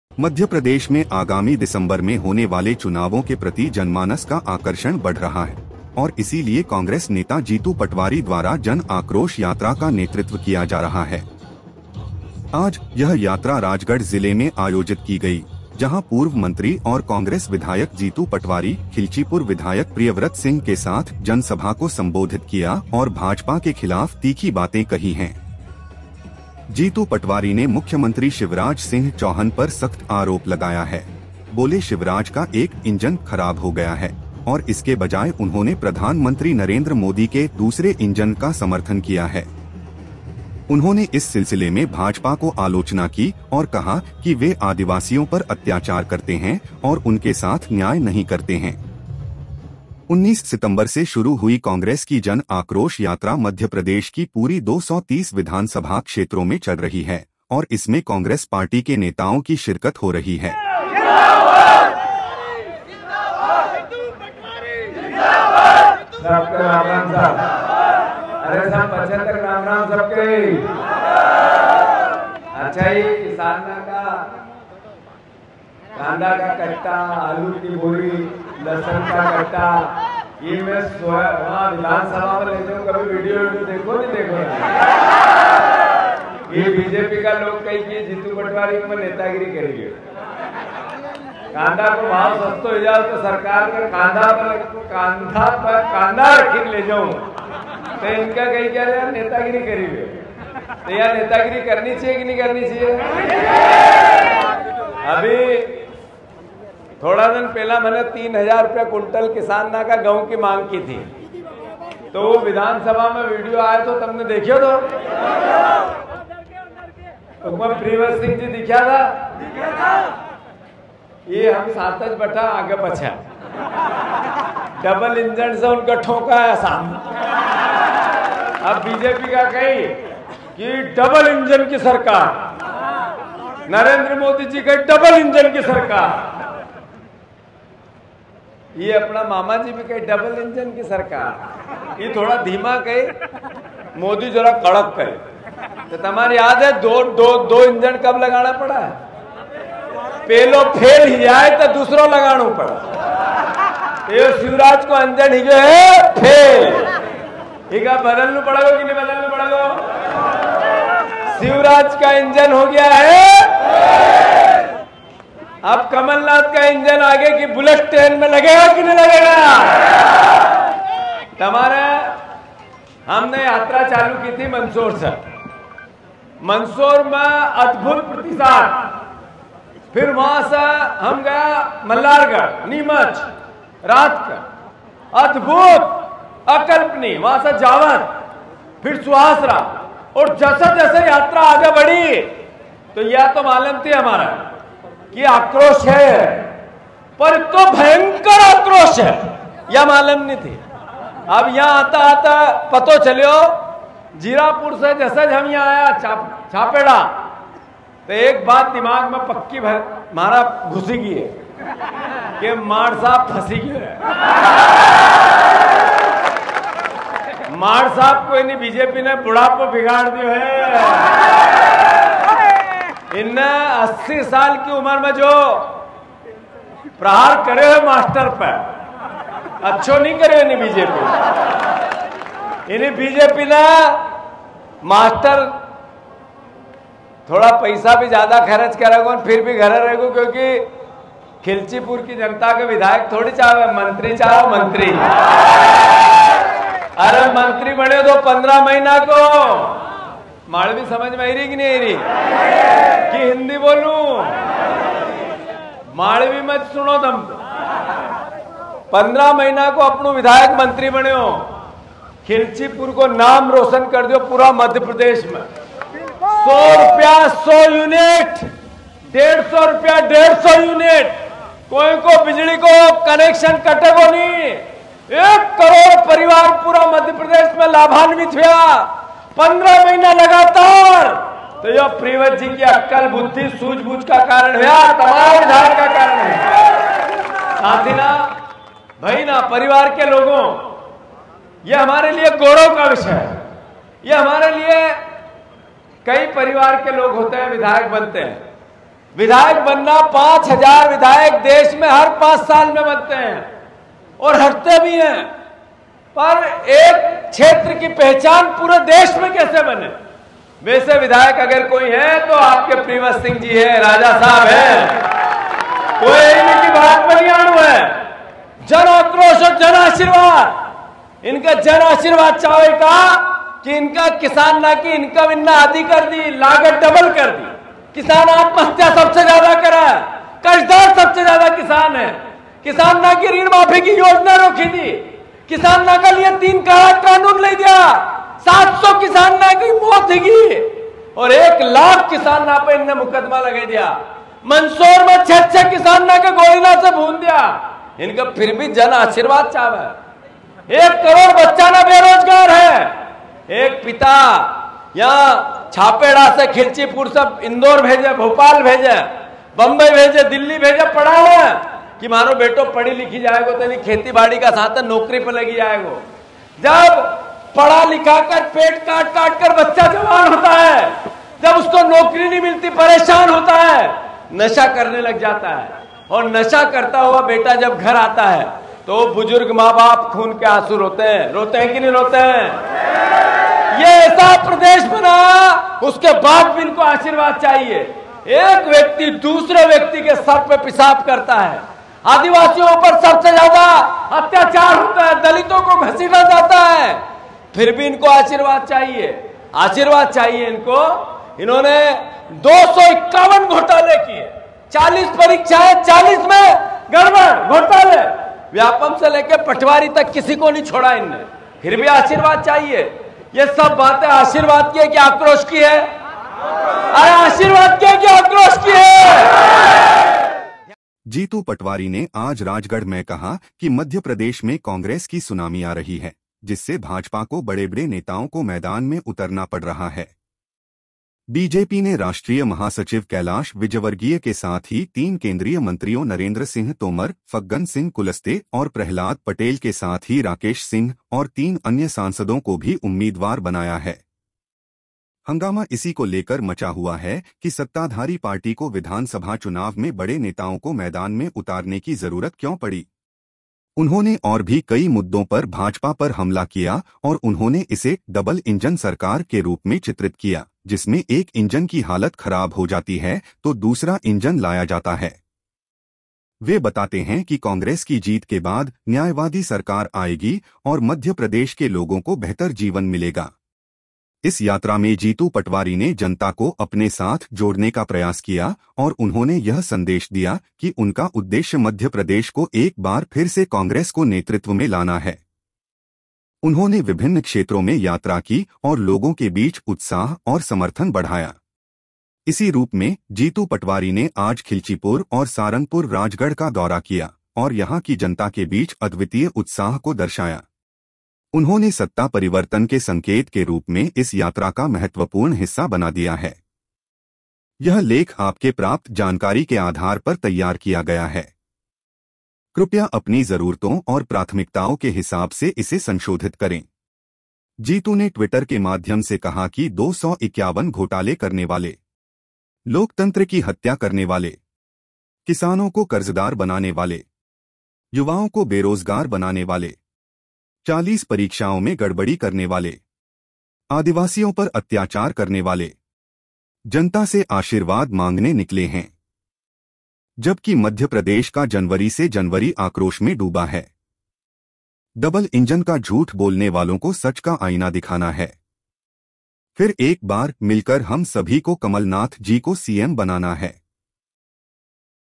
राजगढ़ पहुँची जीतू पटवारी की जन आक्रोश यात्रा, रोड शो के दौरान उमड़ा भारी जनसैलाब | MP Elections 2023
मध्यप्रदेश में आगामी दिसम्बर में होने वाले चुनावों के प्रति जनमानस का आकर्षण बढ़ रहा है, और इसीलिए कांग्रेस के नेता जीतू पटवारी द्वारा जन आक्रोश यात्रा का नेतृत्व किया जा रहा है। आज, यह यात्रा मध्यप्रदेश के खिलचीपुर, सारंगपुर और राजगढ़ जिले में आयोजित की गई, जहां पूर्व मंत्री और कांग्रेस विधायक जीतू पटवारी और दिग्गज नेता प्रियव्रत सिंह जी ने जनसभा को संबोधित कर, भाजपा के खिलाफ तीखी बातें कही हैं। राजगढ़ पूर्व मुख्यमंत्री दिग्विजय सिंह का गढ़ माना जाता है !

राजगढ़ में कांग्रेस MLA जीतू पटवारी की हुंकार, जन आक्रोश यात्रा में उमड़ा भारी जनसैलाब | MP Elections

MP Elections 2023: Congress MLA Jitu Patwari Holds a MASSIVE Political Rally in Khilchipur, Madhya Pradesh